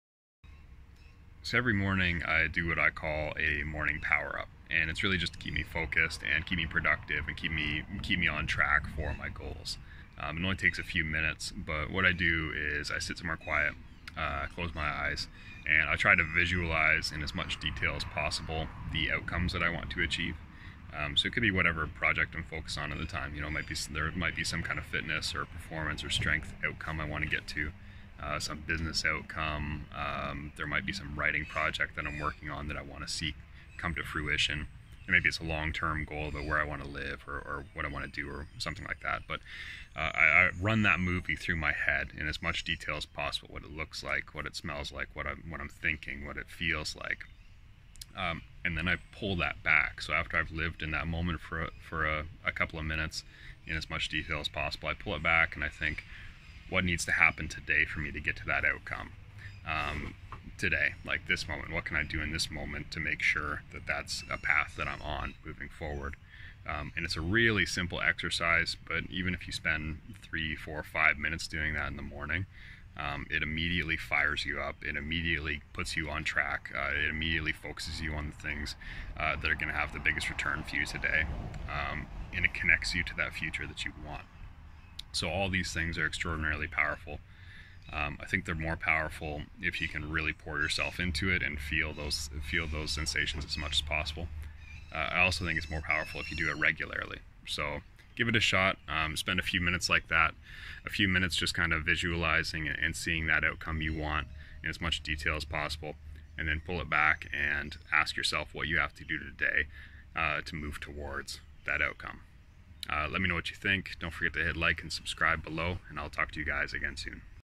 Morning power up!
A morning routine is probably the best investment you can make. Set yourself up for success and the rest of the pieces will fall into place much easier!

More like this in my new book, "Good Vibes Only: why the good vibes are gone, and how to get them back".